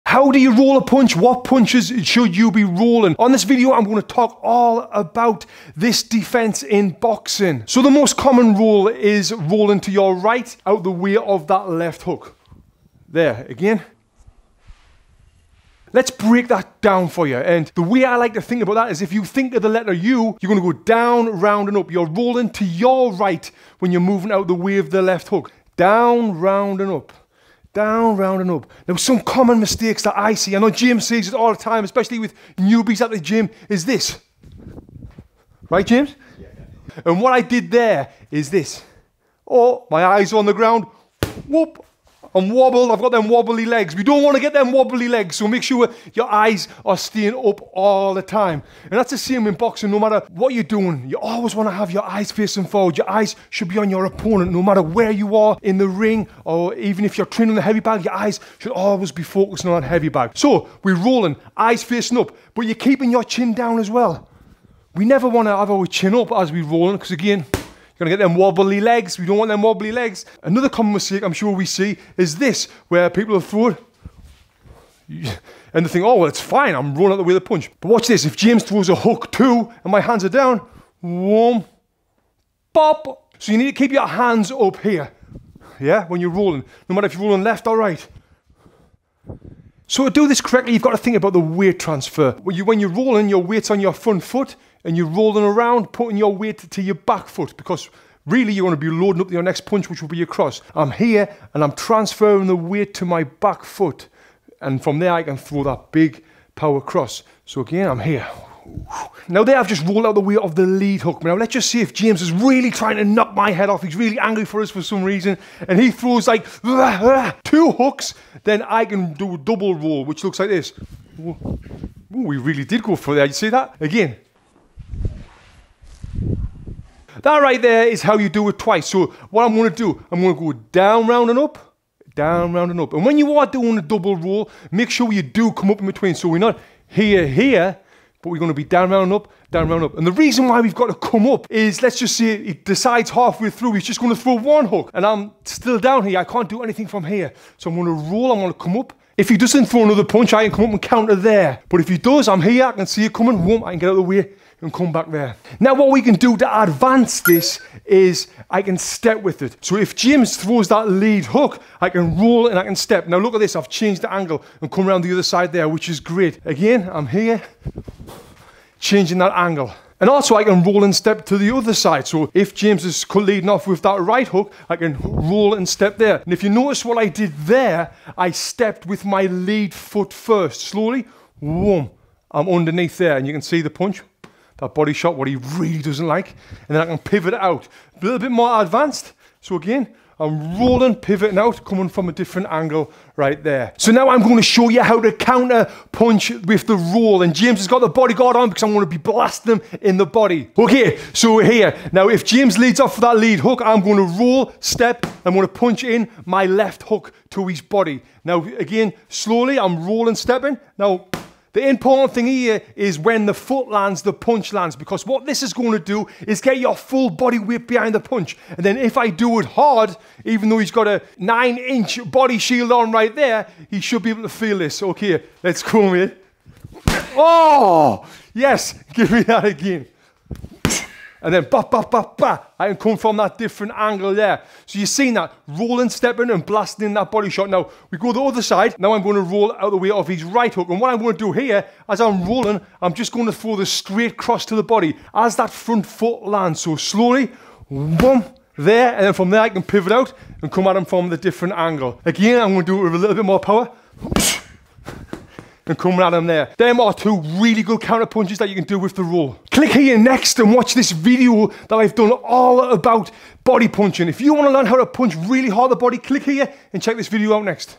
How to Roll a Punch in Boxing | Defense 101
Tony Jeffries on the complete training of how to roll a punch in boxing. If you just started to learn how to box, you will need to know the basic boxing defense techniques. Some of the techniques you need to do are: Don't lower your guards, Keep your Chin down and Look at your Opponent.

Chapters:
0:00 How to Roll Punches in Boxing
1:30 Weight Transfer
1:51 Double Roll
2:35 Advanced Technique
3:14 How to Roll and CounterPunch